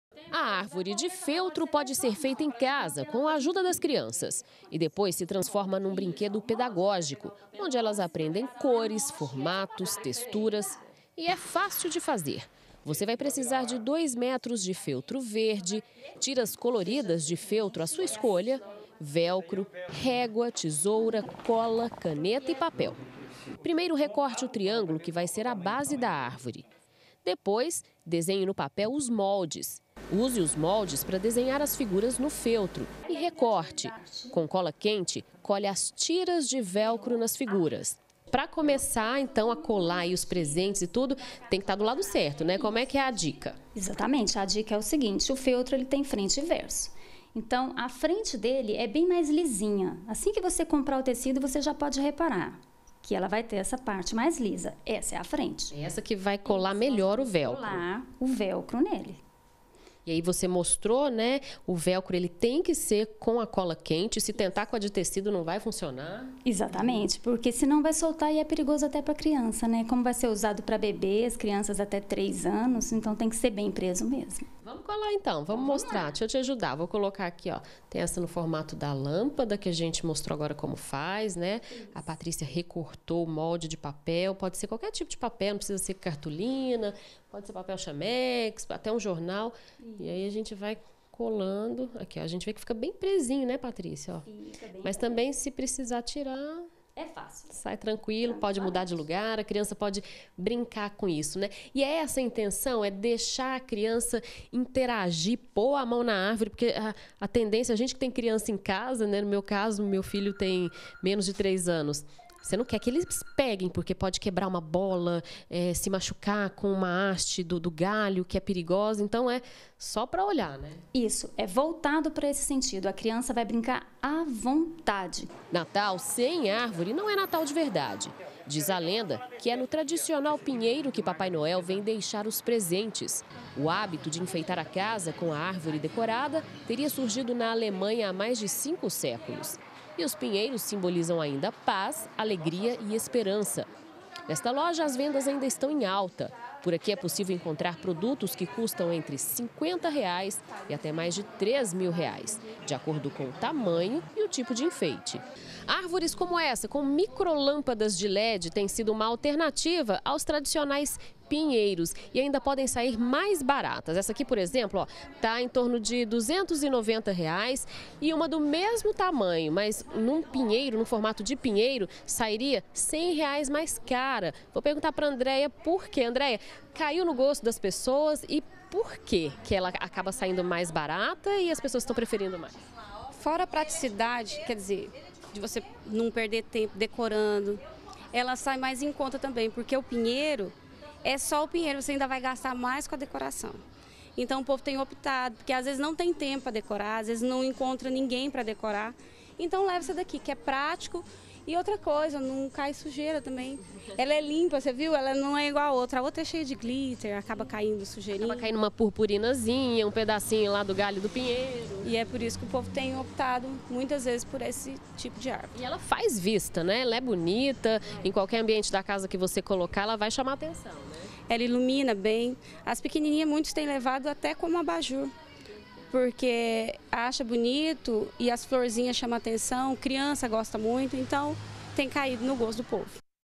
GA - Novos modelos de árvores de natal - 04-12-2017
O natal está chegando e é claro que não pode faltar a tradicional árvore. E tem de todo tipo e modelo, pra comprar e também fazer em casa. A repórter Mônica Novaes foi conferir as novidades.